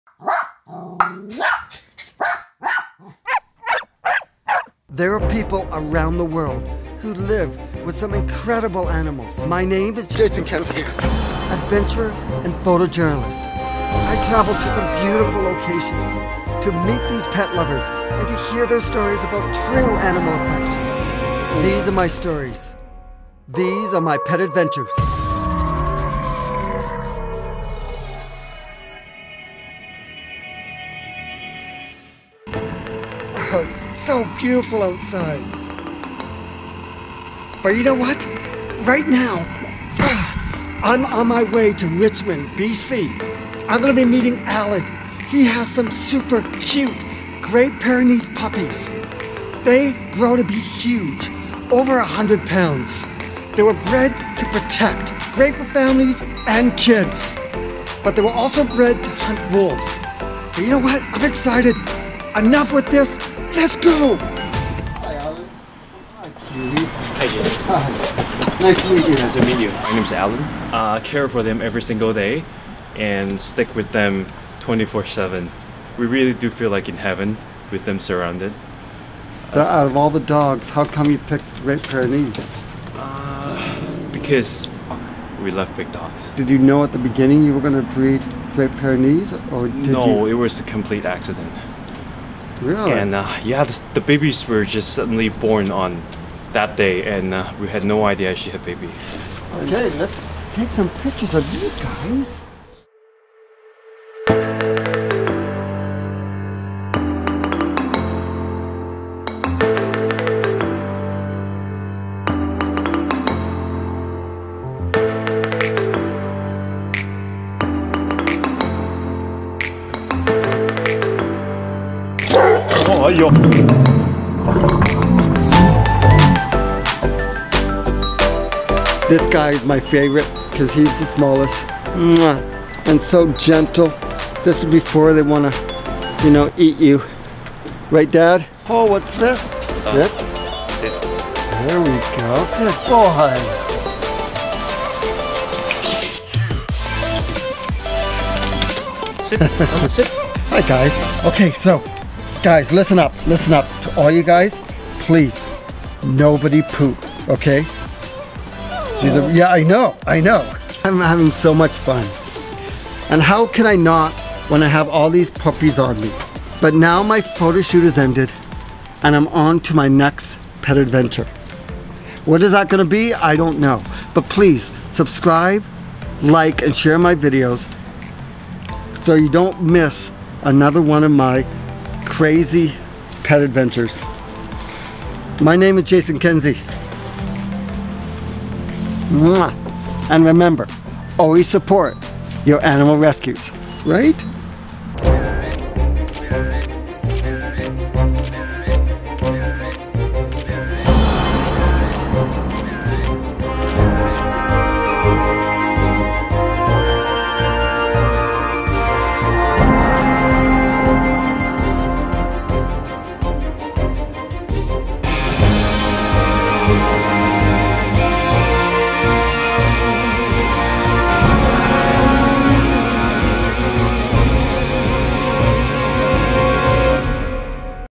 Pyrenees puppies caught kissing each other (Cute overload)
The Photo Warrior Jason Kenzie Photographs Great Pyrenees puppies